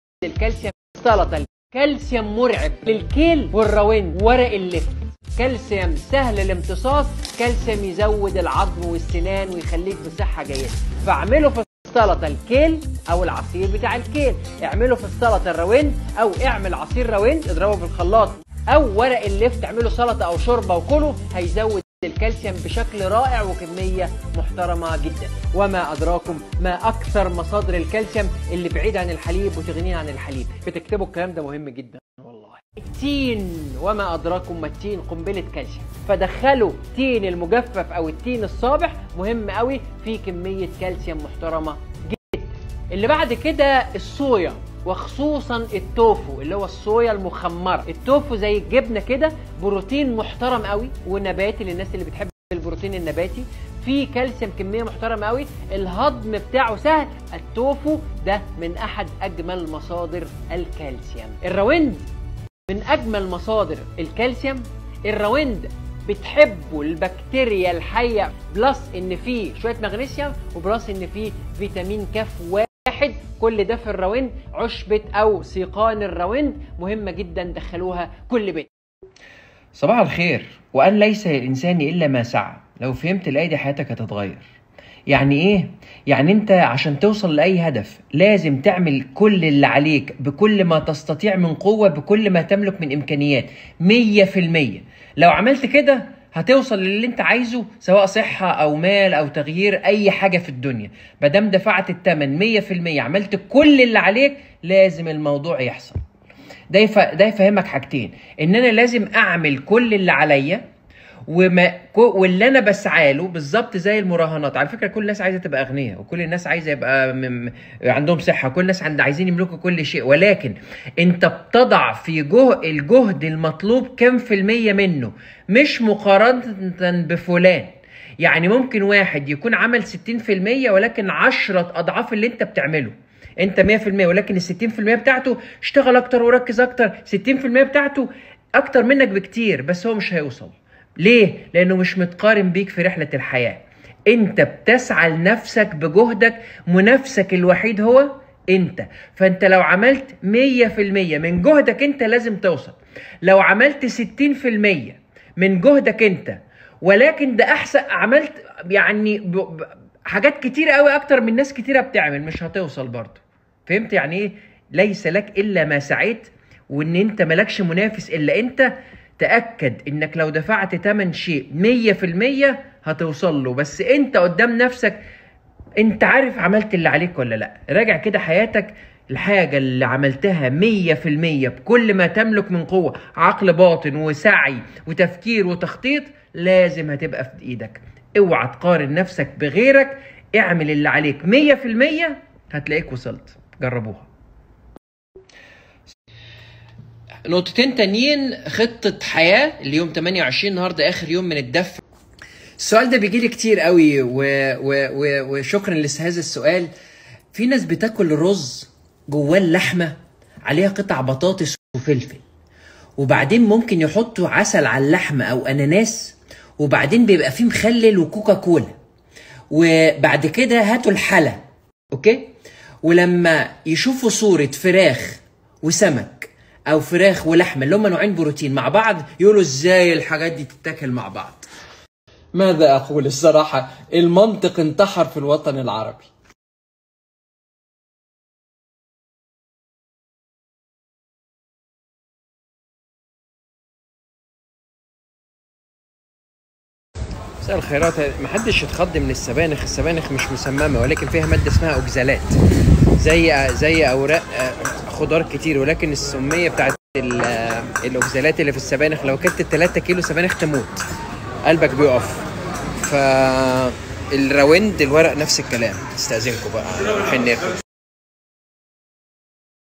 اهم مصادر الكالسيوم+سمية السبانخ والراوند/الدكتور كريم على
افضل وقت لتناول الكولاجين +التدريب في الجم مساًء+استأصال المراره والكركم+انقاص الوزن